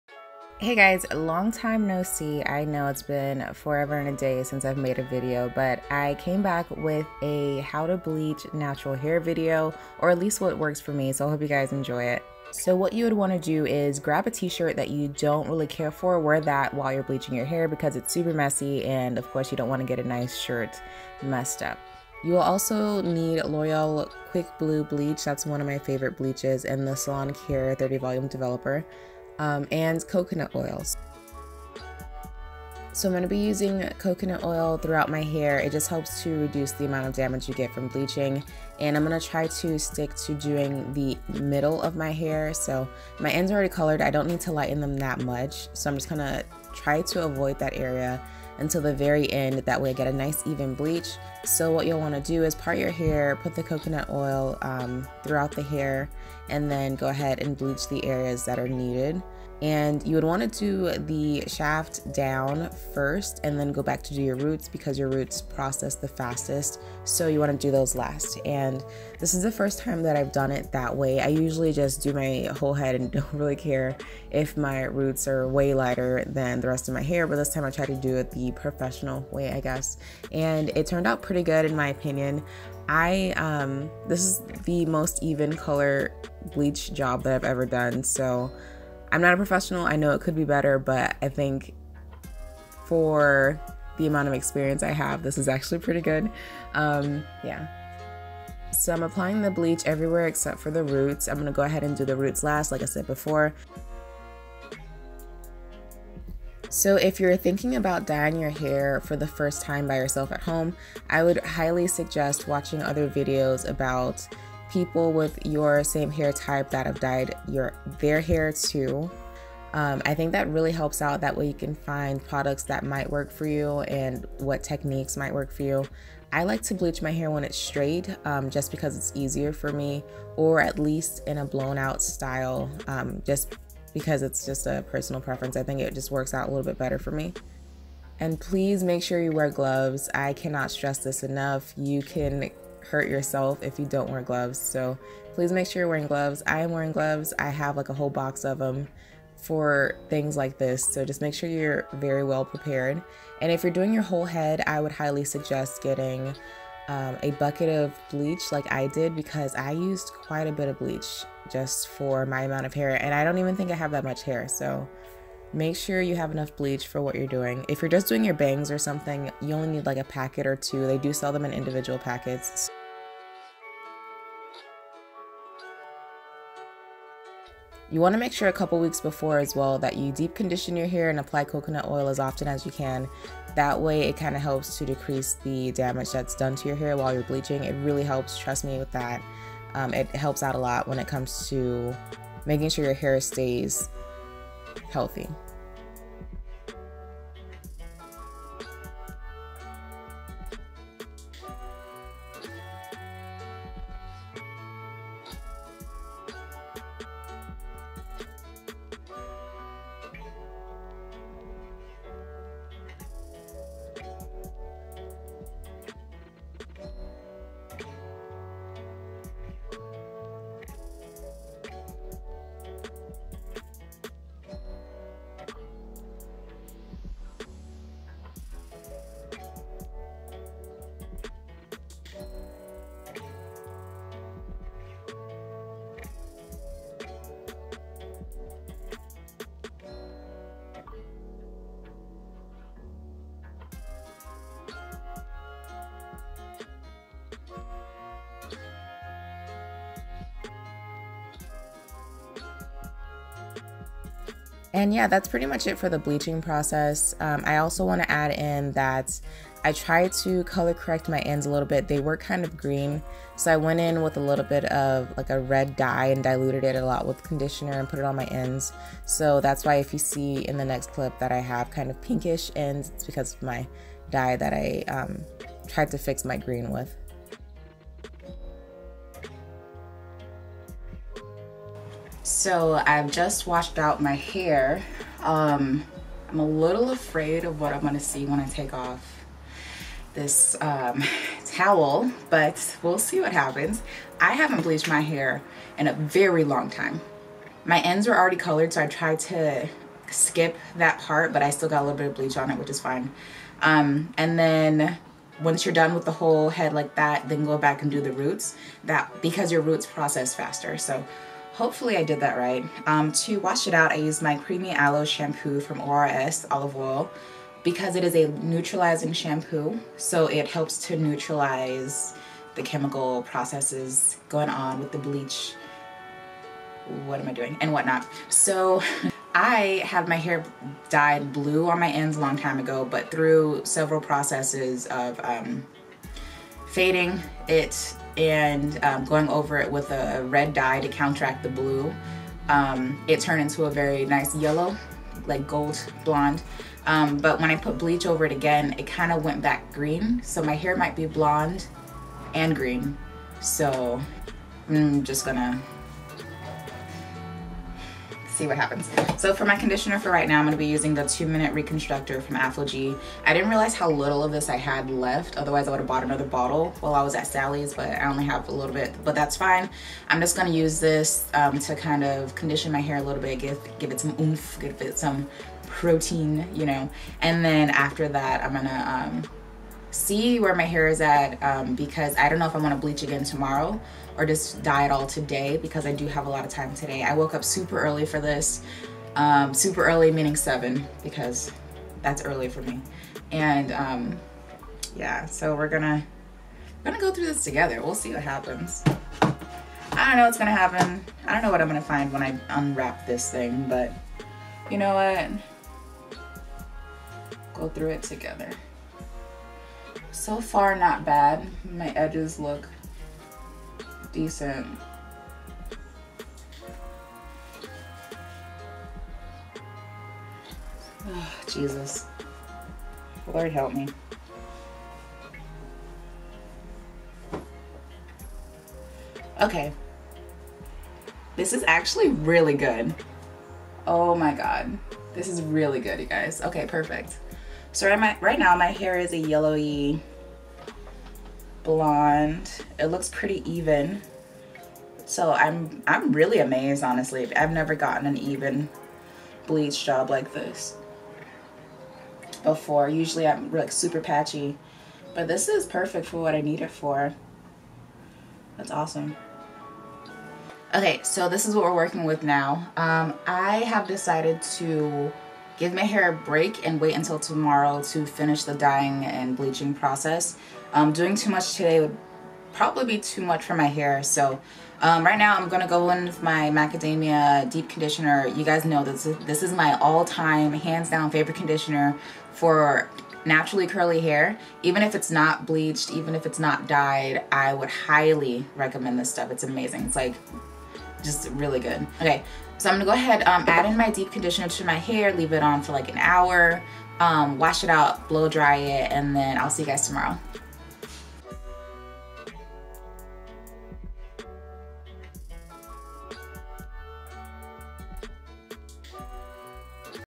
I Bleached My Hair!!!
Hello everyone long time no see! Today I will be showing you how I bleach my natural hair. This is not exactly a tutorial, I'm just showing you what works for me. Hair bleaching is NO JOKE!!! Please be careful and do your research before dying or bleaching your hair.

Add Me! :)

Week Before:

Deep Condition / Protein treatment
Put Coconut Oil throughout hair, focus on ends

What you’ll need:
L'oreal Quick Blue Bleach
Salon perfect 30 vl creme developer
Coconut Oil
Old T-Shirt
Mixing Bowl(s)
Gloves
Color Corrector / Toner
Hair Clips / Hair ties
ORS Neutralizing shampoo
Deep Conditioner / Macadamia Deep Conditioner
Super Skinny serum / Heat Protectant